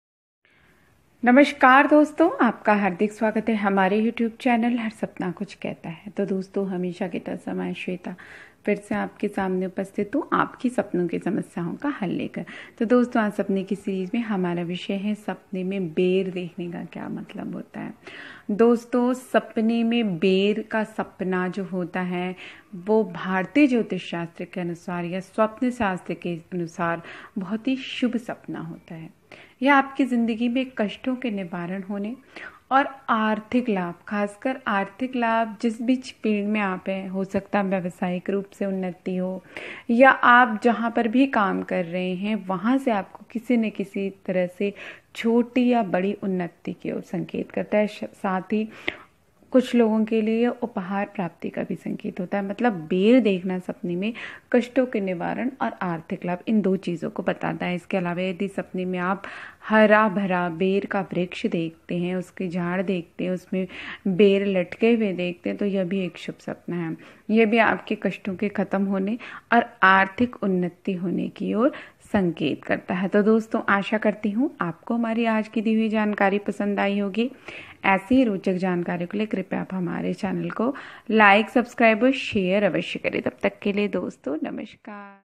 सपने में बेर देखना. Sapne me ber fruit dekhna.
Astrological  interpretations  of  seeing  ber fruit  in  dreams.